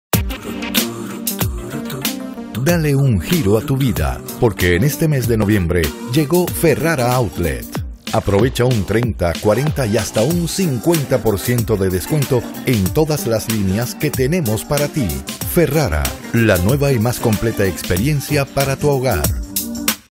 Volvió Ferrara Outlet - noviembre 2016
¡noviembre tiene grandes sorpresas porque es el mes de FerraraOutlet! Consiéntete y remodela tu hogar con un descuento en mercancía seleccionada que no te puedes perder. 30%, 40% y un 50% sólo por el mes de noviembre 2016 ¡Ven y deléitate!